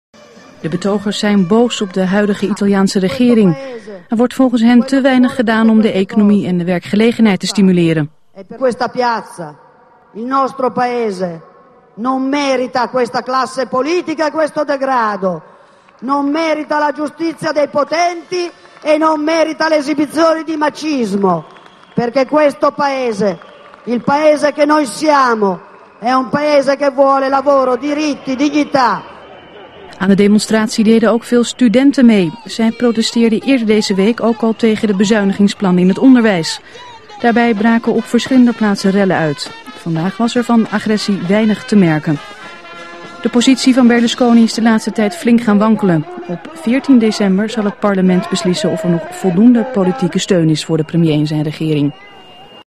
Italianen zijn dictator Berlusconi meer dan zat
Tienduizenden mensen zijn in Rome de straat op gegaan om te demonstreren tegen de regering van premier Berlusconi. Er wordt volgens hen te weinig gedaan om de economie en de werkgelegenheid te stimuleren.